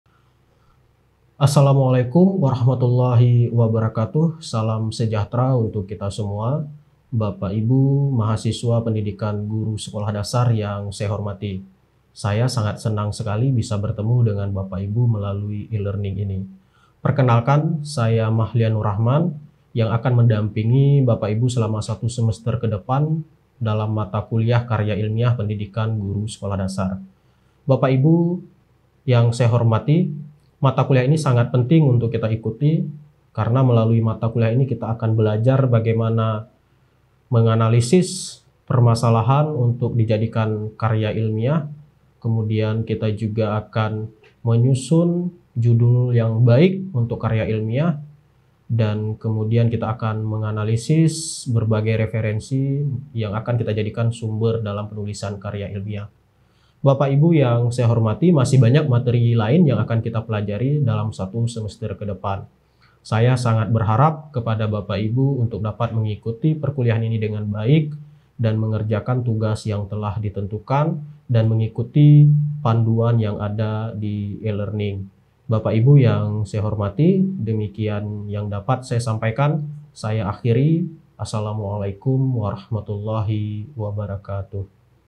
KARYA ILMIAH PENDIDIKAN GURU SEKOLAH DASAR
Video ini digunakan sebagai video perkenalan untuk mata kuliah Karya Ilmiah Pendidikan Guru Sekolah Dasar pada Program Studi Pendidikan Guru Sekolah Dasar, Universitas Terbuka.